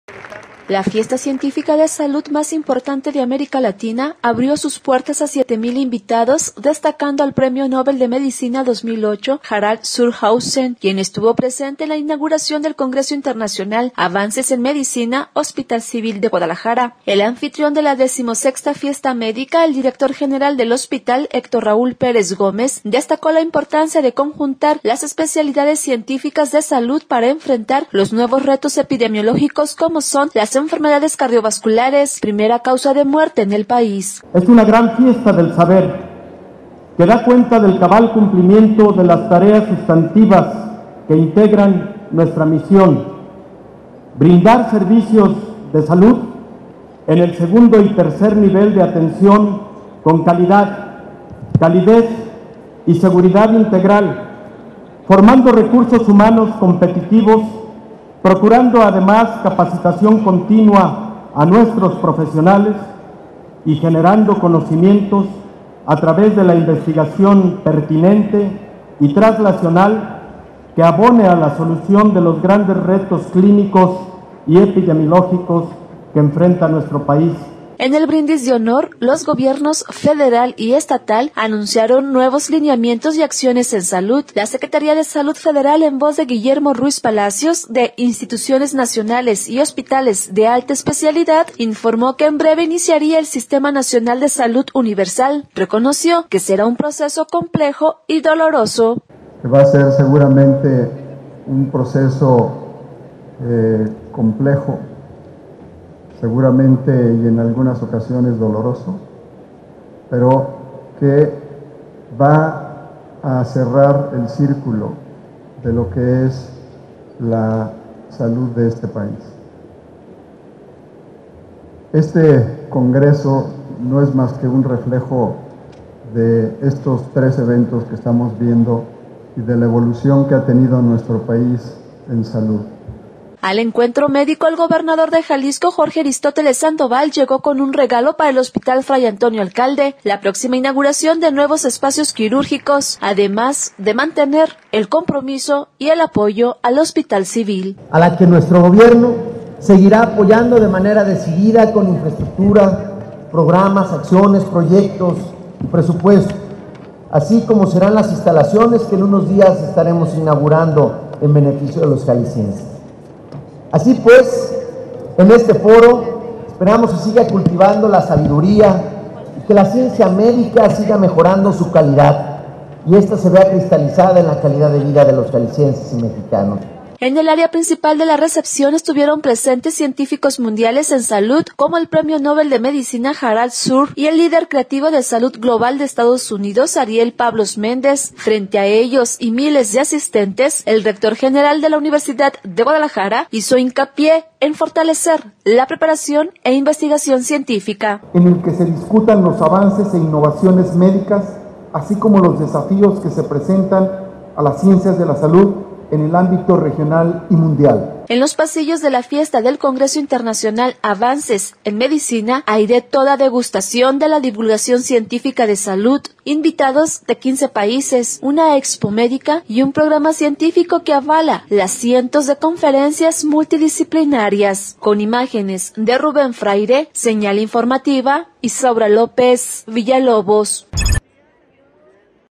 Señal Informativa: Consolidará gobierno federal el Sistema de Salud Universal
En la fiesta de la medicina Hospital Civil de Guadalajara el gobierno federal anunció que en breve se estará consolidando el Sistema Nacional de Salud Universal, reconoce el representante de la Secretaria de Salud Guillermo Ruíz, que será doloroso pero cerrará el círculo para atender con calidad a los mexicanos mientras que el gobierno estatal asume el compromiso de seguir apoyando al organismo público descentralizado.

Informa: Isaura López Villalobos